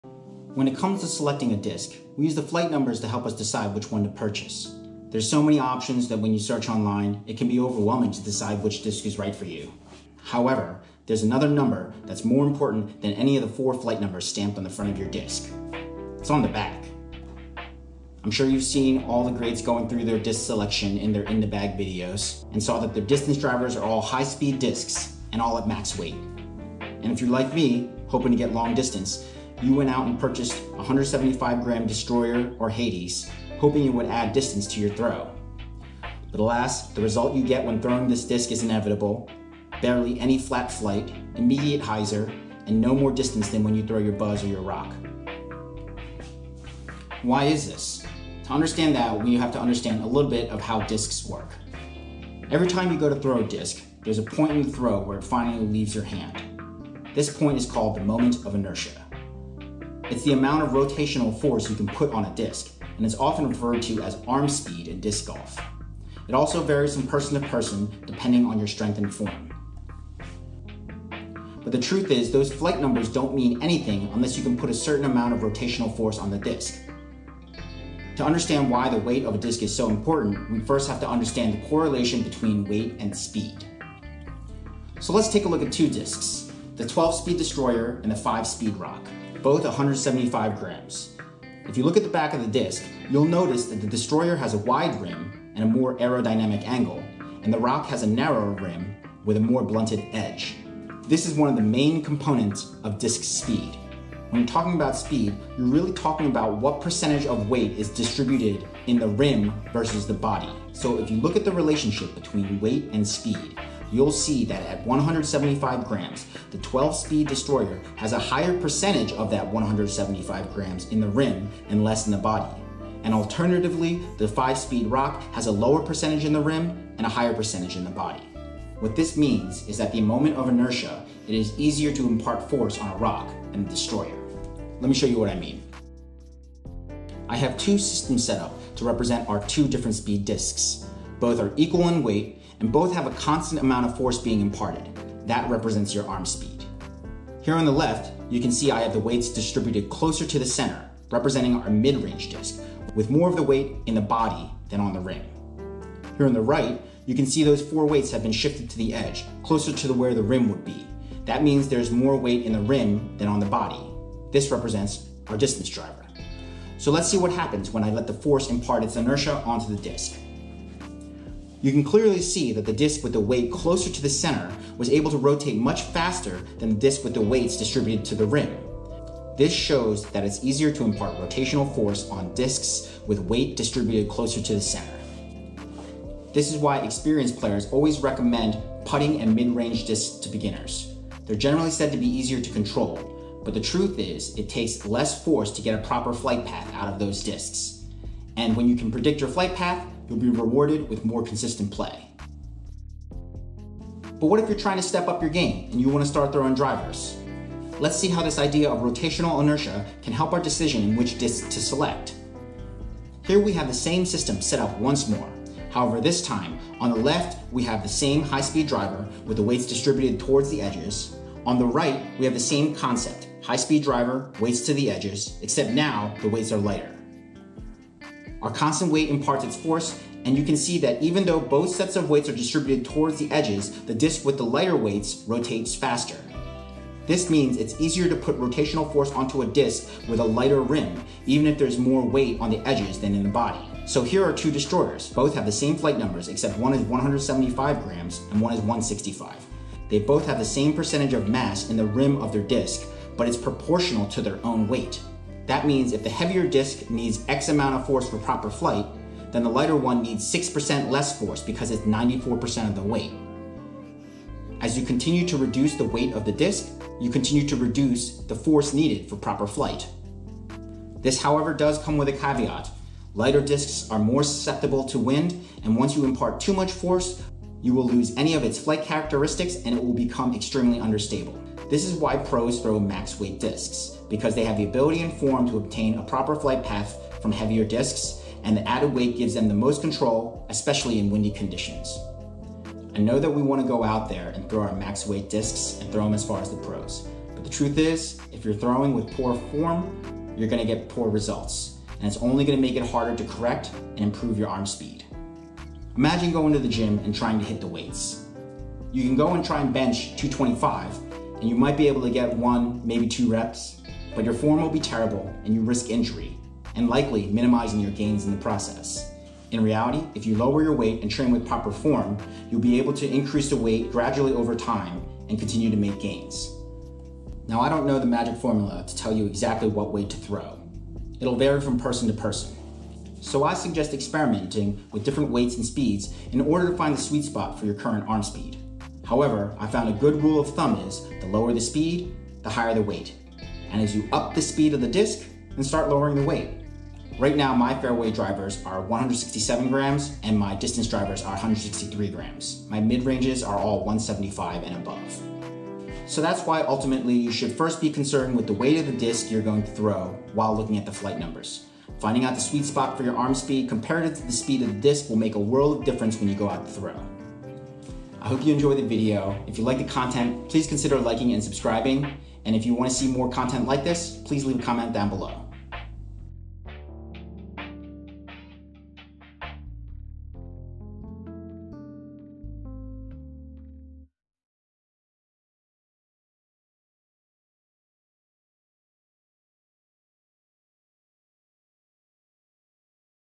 What is the most IMPORTANT flight rating number for most distance?
Here's a quick video where I talk about why the weight of the disc can be more important that any of the flight ratings.

Hoping this information might help some beginners trying to throw drivers.

I'm not a physicist, but thinking about the concepts of putting rotation on the disc really changed how I approached throwing.